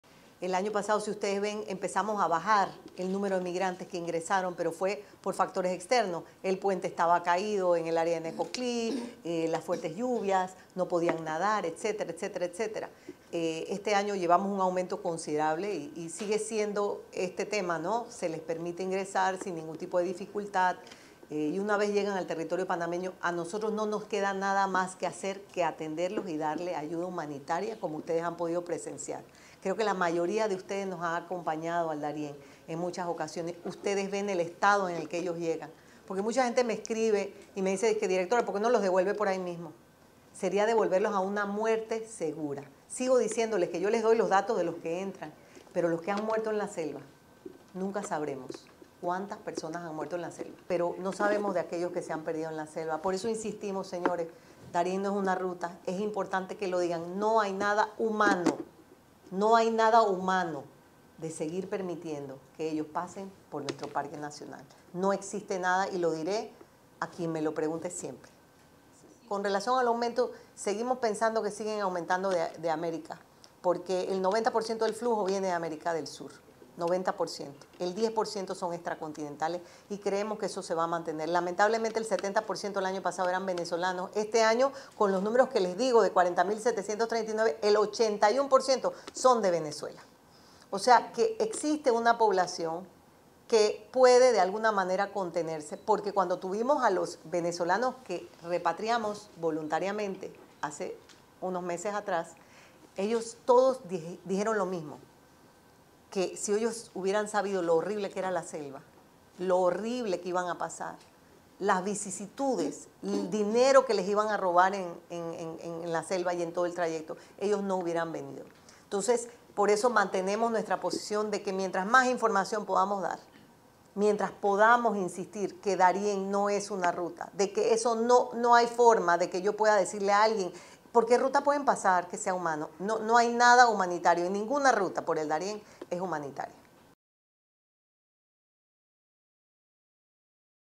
Migración irregular aumentará 20% en 2024
Solo en el primer mes de este año, 39 mil personas pasaron la frontera entre Colombia y Panamá